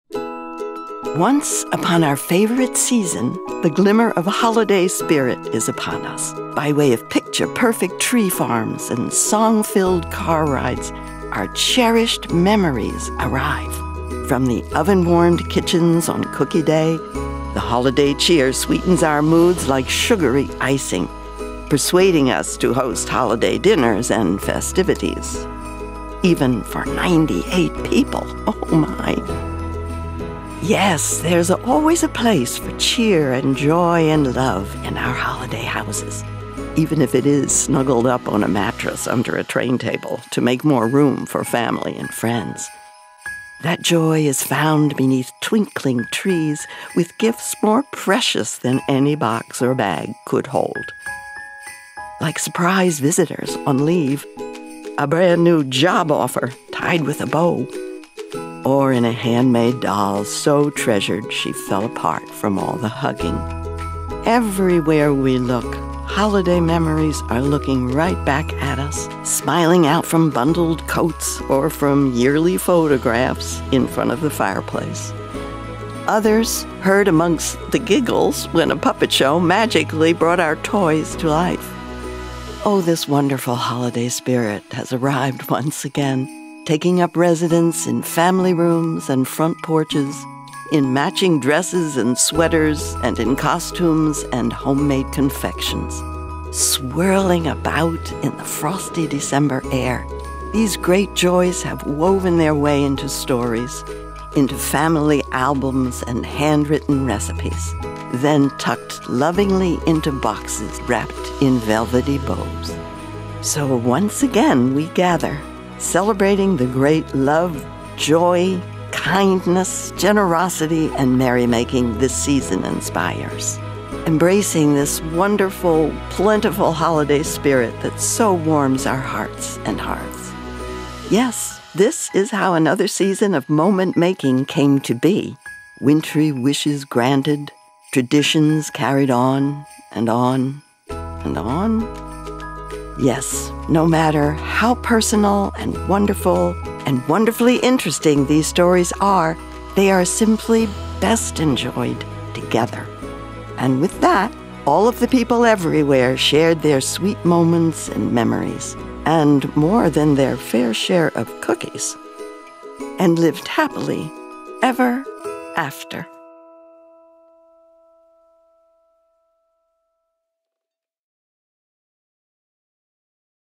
Westfield 2019 Holiday Video
This year’s video highlights favorite holiday memories our Westfield team shares with family and friends. And embraces each of our strengths and perspectives. Each view of the holiday video increases our donation to National VOAD.

From Catholic Charities to Islamic Relief, National Voluntary Organizations Active in Disaster (National VOAD) brings together all faith-based and secular organizations across the U.S. who are dedicating to helping survivors of disaster, so they can return home, return to work and return to a state of normalcy.